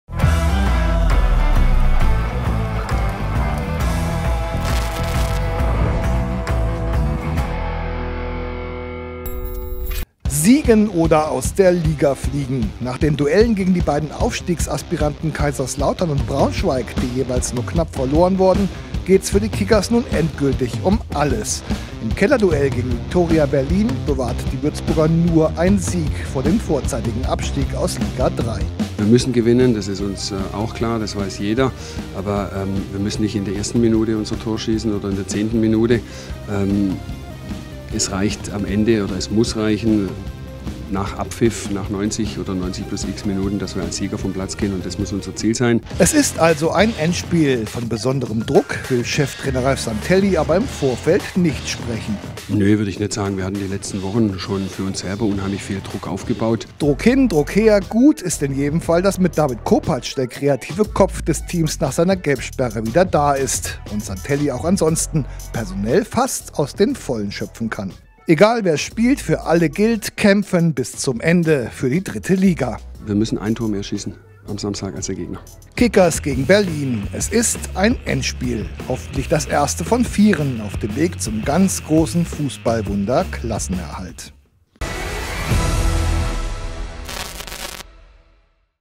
Kickers TV: Die Vorschau auf das Heimspiel gegen Viktoria Berlin
"Wir müssen gewinnen!" Die Devise für das Heimspiel gegen Viktoria Berlin ist klar. Nur ein Sieg hält die Chance auf den Klassenerhalt aufrecht. Mit welcher personellen und welcher Stimmungslage die Rothosen in die Partie gehen, seht Ihr im Klubfernsehen.